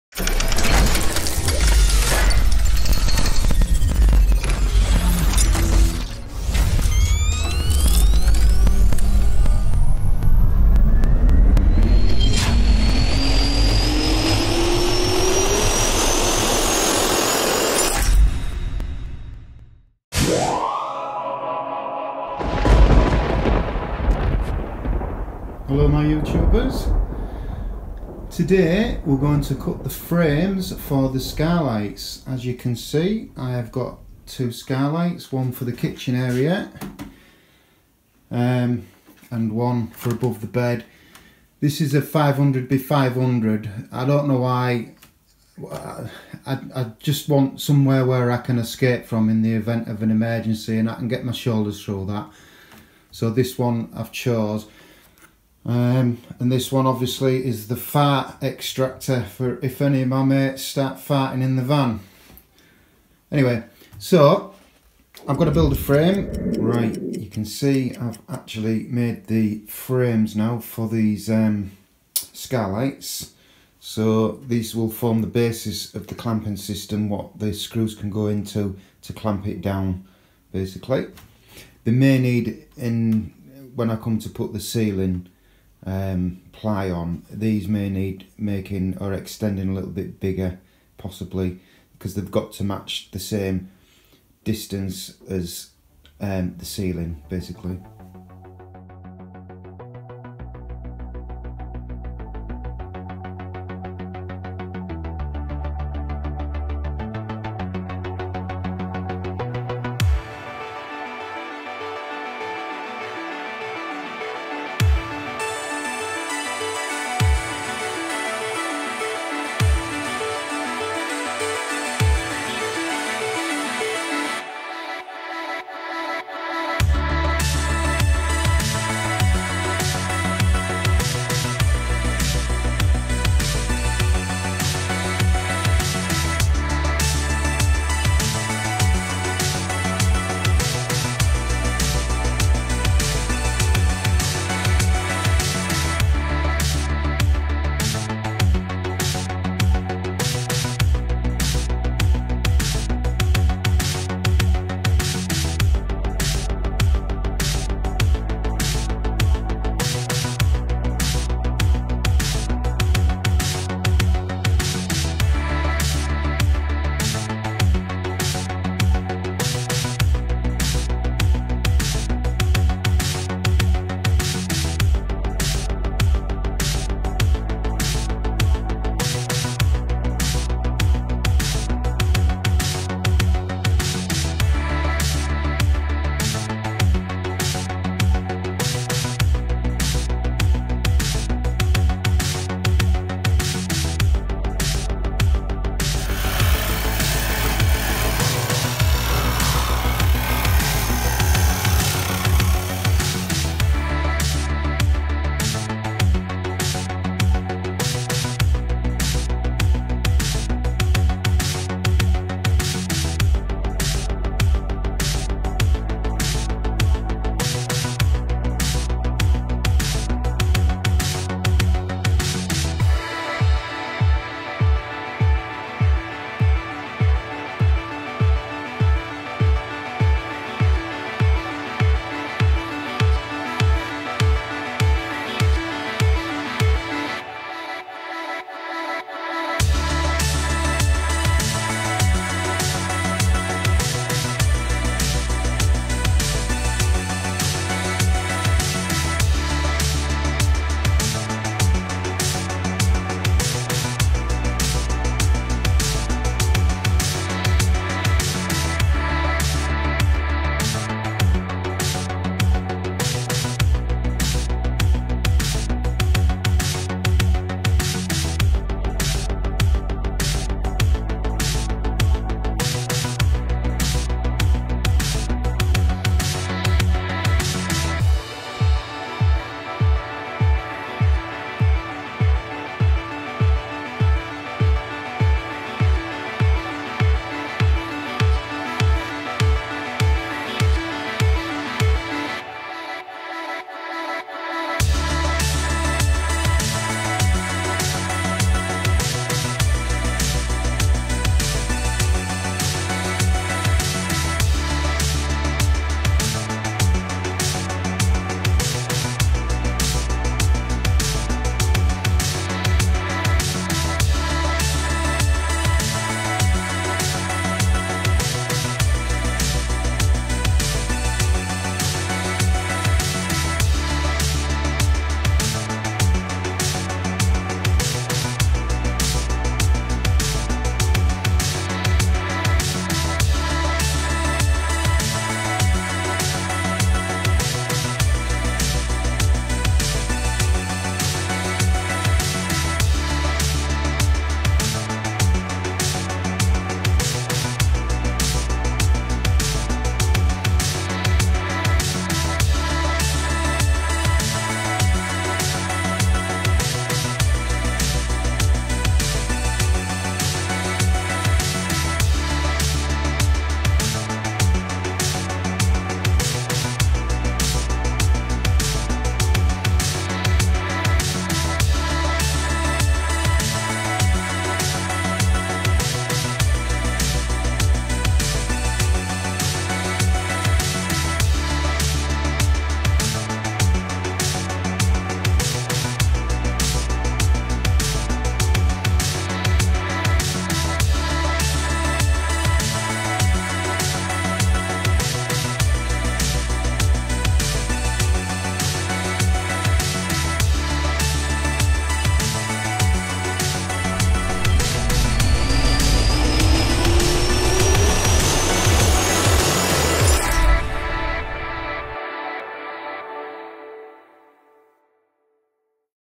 Fitting Skylights in my Camper
Watch in time lapse the one job I’ve being dreading because if it ain’t right, it could leak or even worse won’t fit!